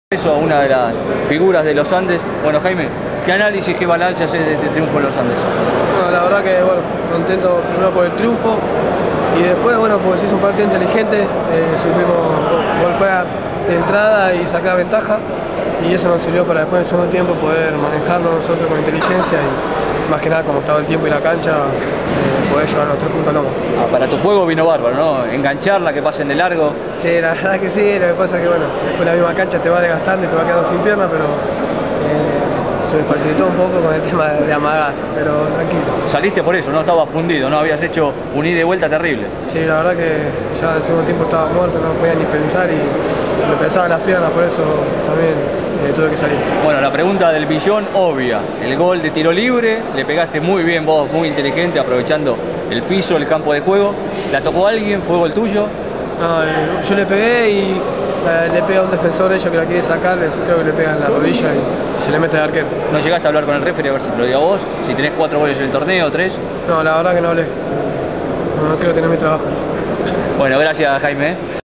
Jaime Molfeso
Nota a Jaime Molfeso luego del partido entre Atlanta (1) y Los Andes (3) bajo la lluvia.